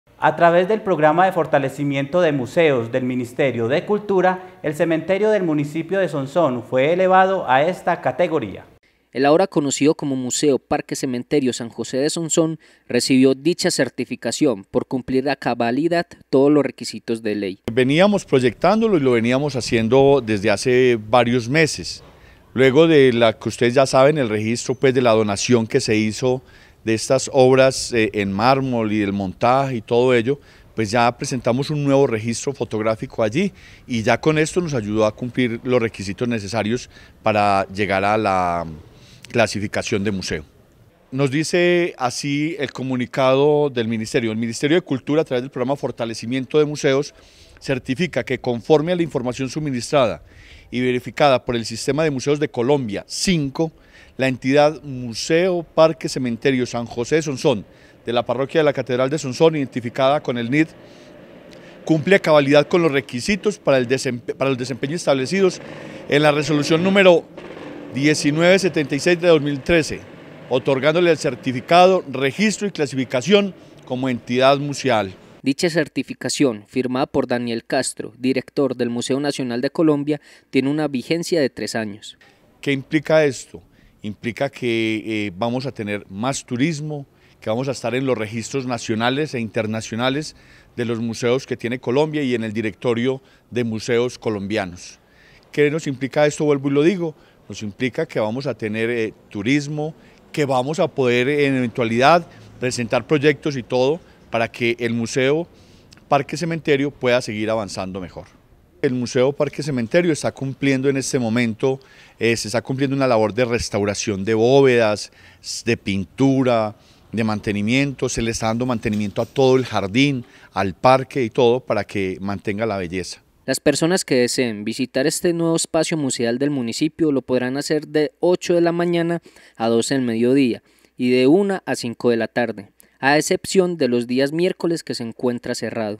CEMENTERIO DE SONSÓN CERTIFICADO COMO ENTIDAD MUSEAL
A través del programa de fortalecimiento de museos del Ministerio de la Cultura de Colombia, el cementerio del municipio de Sonsón fue elevado a esta categoría.